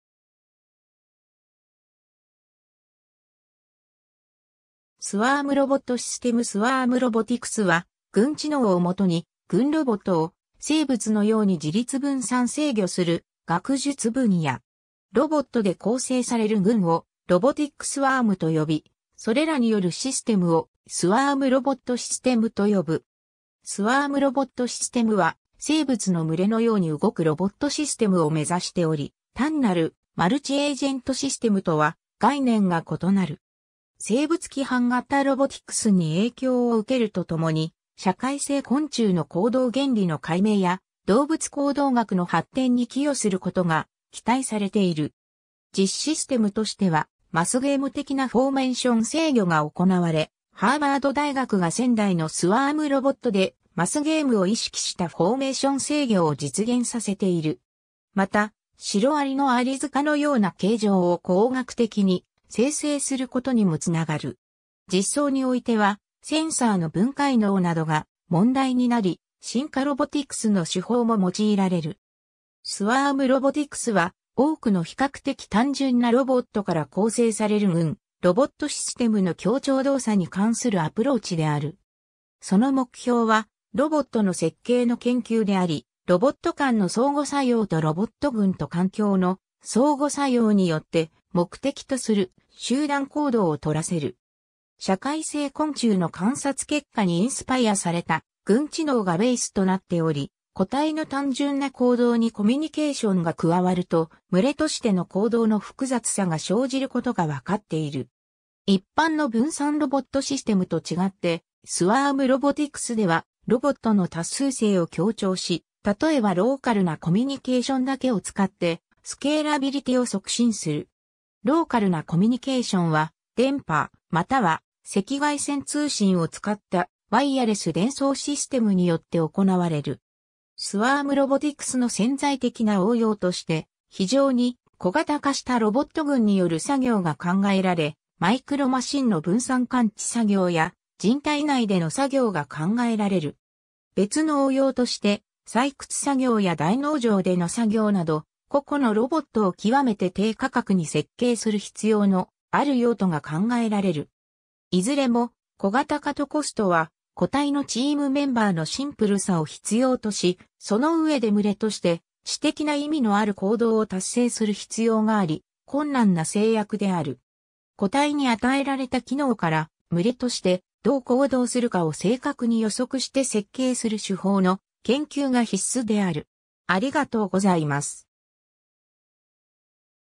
スワームロボティクス
スワームロボットシステム スワームロボティクス（Swarm Robotics、SR）は、群知能（Swarm Intelligence、SI）をもとに群ロボットを生物のように自律分散制御する学術分野。
ロボットで構成される群をRobotic swarm（ロボットスワーム、ロボティックスワーム）と呼び、それらによるシステムをスワームロボットシステム（Swarm Robot System、SRS）と呼ぶ。
スワームロボットシステムは生物の群れのように動くロボットシステムを目指しており、単なるマルチエージェントシステムとは概念が異なる。
生物規範型ロボティクスに影響を受けるとともに、社会性昆虫の行動原理の解明や動物行動学の発展に寄与することが期待されている。
実システムとしてはマスゲーム的なフォーメンション制御が行われ、ハーバード大学が1000台のスワームロボットでマスゲームを意識したフォーメーション制御を実現させている。
また、シロアリのアリ塚のような形状を工学的に生成することにもつながる。
実装においてはセンサーの分解能などが問題になり、進化ロボティクスの手法も用いられる。
 スワームロボティクスは多くの比較的単純なロボットから構成される群ロボットシステムの協調動作に関するアプローチである。
その目標はロボットの設計(物理的な設計と実装すべき振る舞いの設計)の研究であり、ロボット間の相互作用とロボット群と環境の相互作用によって目的とする集団行動をとらせる。
社会性昆虫の観察結果にインスパイアされた群知能がベースとなっており、個体の単純な行動にコミュニケーションが加わると、群れとしての行動の複雑さが生じることが分かっている。
 一般の分散ロボットシステムと違って、スワームロボティクスではロボットの多数性を強調し、例えばローカルなコミュニケーションだけを使って、スケーラビリティを促進する。
ローカルなコミュニケーションは、電波または赤外線通信を使ったワイヤレス伝送システムによって行われる。
 スワームロボティクスの潜在的な応用として、非常に小型化したロボット群による作業(ナノマシン、マイクロボット)が考えられ、マイクロマシンの分散感知作業や人体内での作業が考えられる。
別の応用として、採掘作業や大農場での作業など、個々のロボットを極めて低価格に設計する必要のある用途が考えられる。
 いずれも、小形化とコストは、個体のチームメンバーのシンプルさを必要とし、その上で群れとして知的な意味のある行動を達成する必要があり、困難な制約である。
 個体に与えられた機能から、群れとしてどう行動するかを正確に予測して設計する手法の研究が必須である。